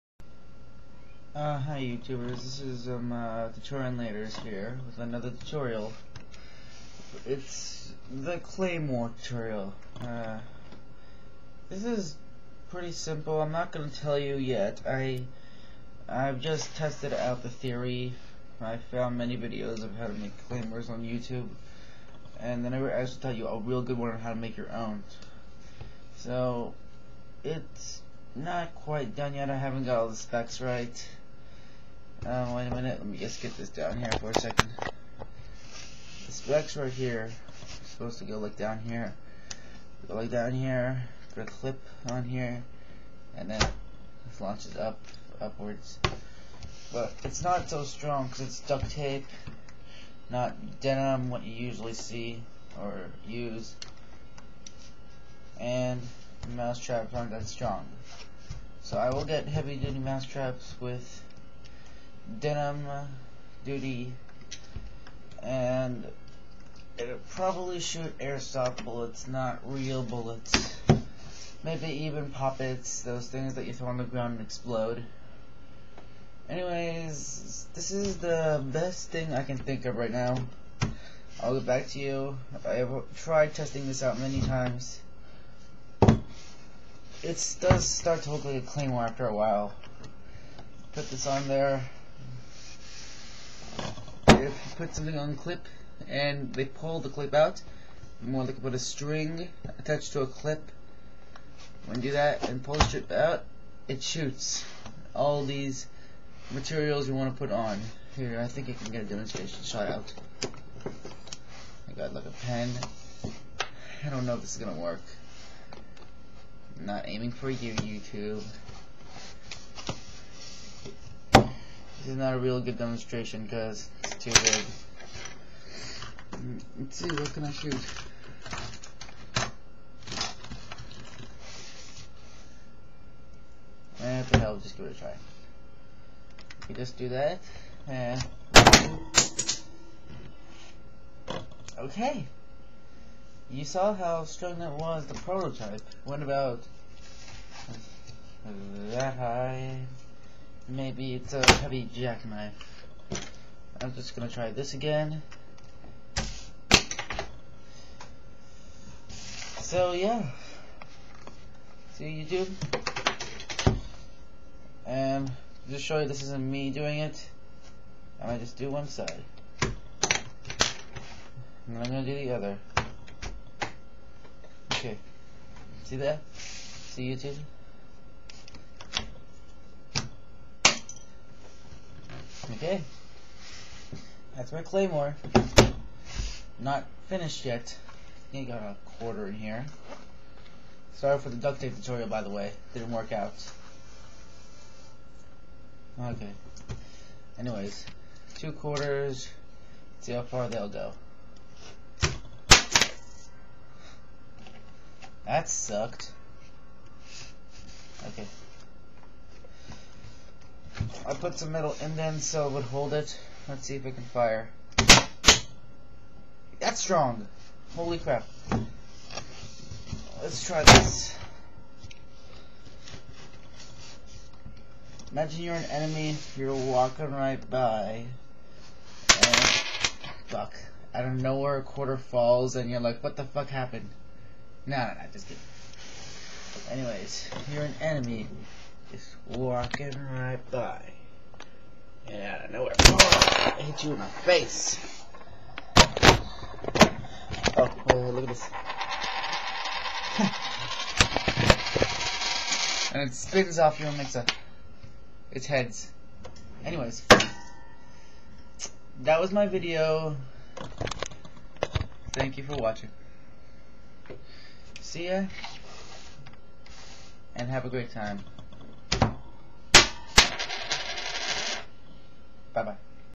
the claymore preveiw and test of the home made claymore perfected PROTOTYPE
THIS CLAYMORE TUT IS FOUND IN YOUTUBE BUT I DIDNT MAKE THE DESIGN OF THE FIRERING MECH BUT JUST THE TRIGGERING MECH